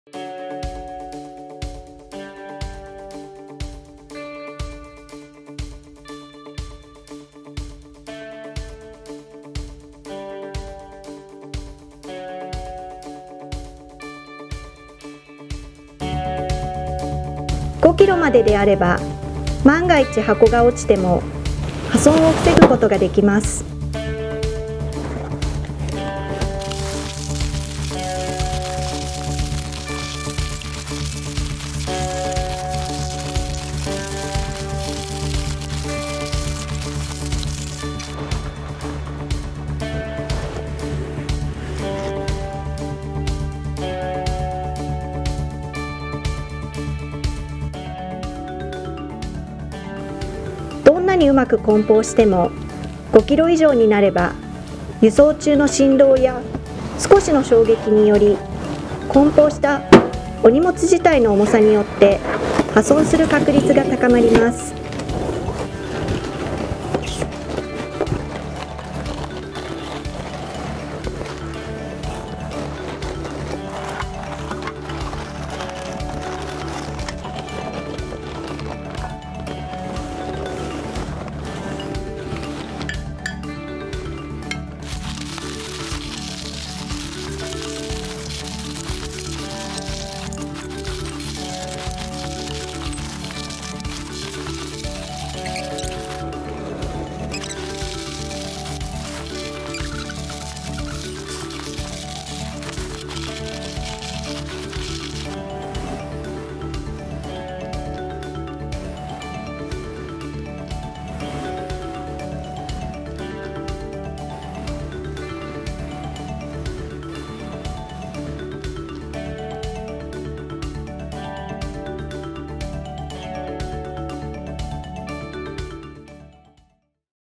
比較検証
箱に５kg以上詰めた場合と５kg未満の場合の、食器の割れやすさを検証しました。